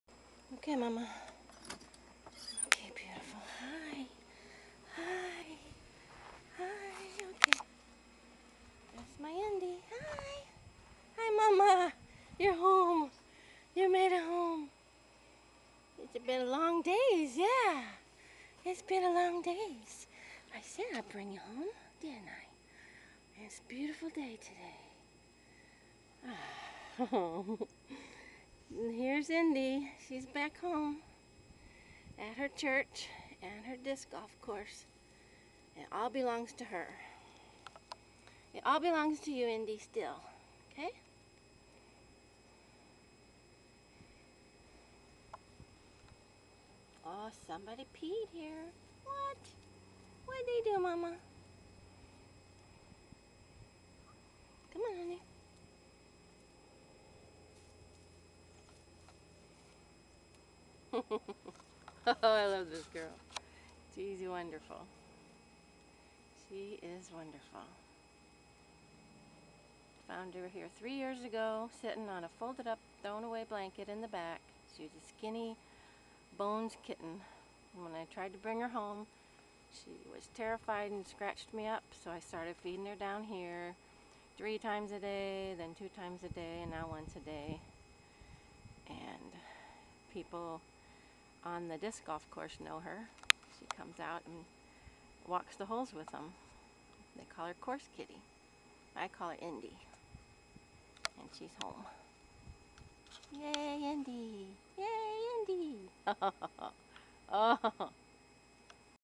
Indie! Return to Home
Indie is a community cat in NC who lives at a church and disc golf! We got some very cold temps this week so I tricked her into a carrier and brought her to my home for 4-5 days til the weather improved. She is so glad to be home! Love love love Indie, Course Kitty, Church Kitty.